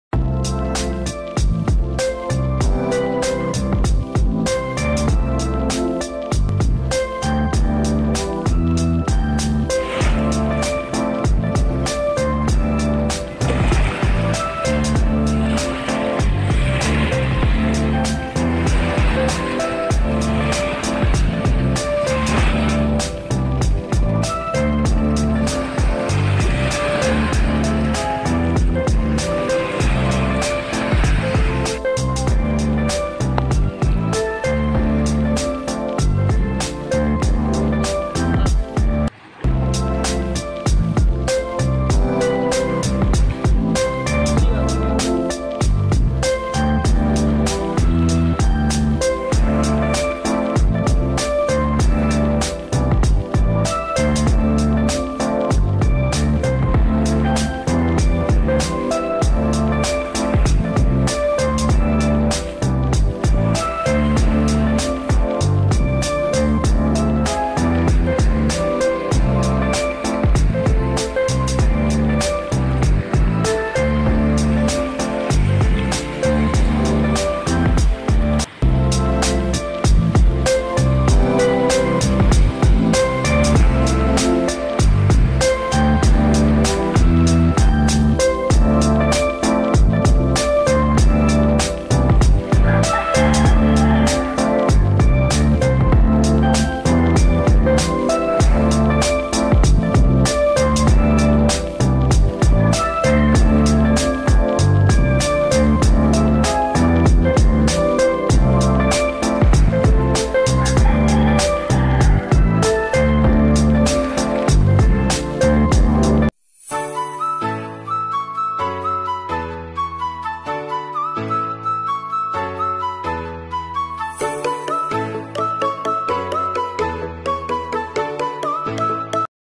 Pahangin sa tabing dagat
In this vedio i will be sharing you guys the beauty of our place where you can have to unwind the serenity of the beach and the view of a sweet breathtaking  heavenly clear clouds that gives the sorrounding more beautiful and serene.